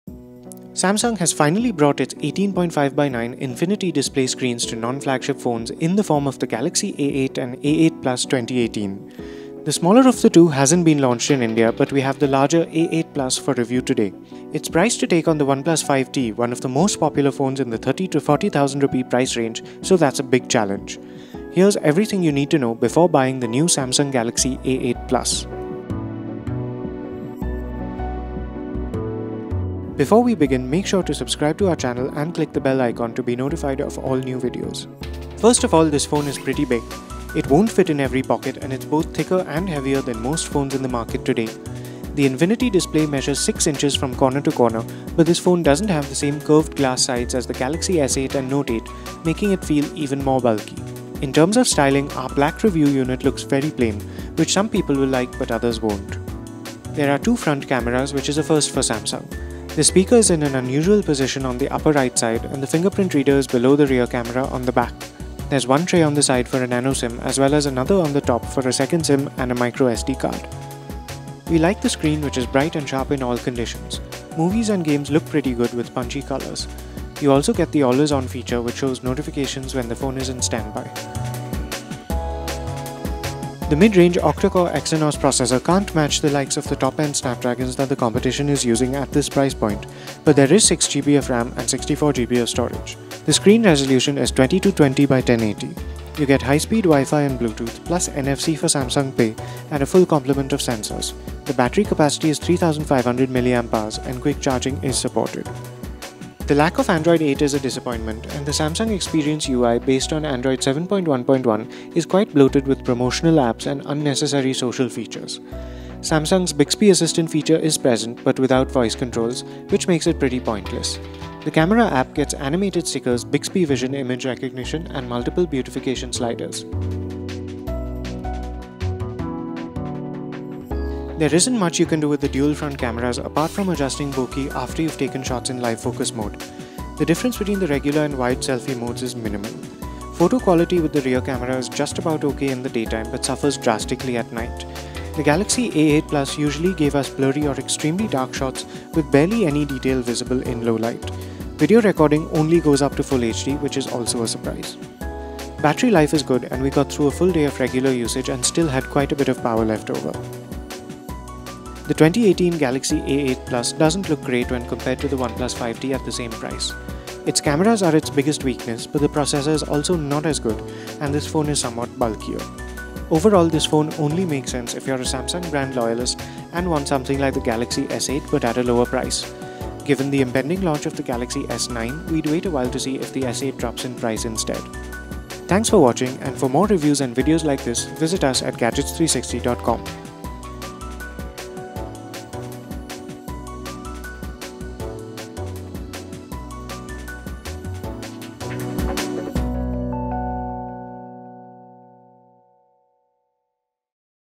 Samsung Galaxy A8+ (2018) Review | Camera, Specs, Verdict, and More
Samsung has finally brought its 18.5:9 Infinity Display screens to non-flagship phones, in the form of the Galaxy A8 and A8+ (2018). The smaller of the two hasn’t been launched in India, but we have the larger A8+ for review today. It’s priced to take on the OnePlus 5T, one of the most popular phones in the Rs. 30-40,000 price range, so that’s a big challenge. Here’s everything you need to know before buying the new Samsung Galaxy A8 Plus.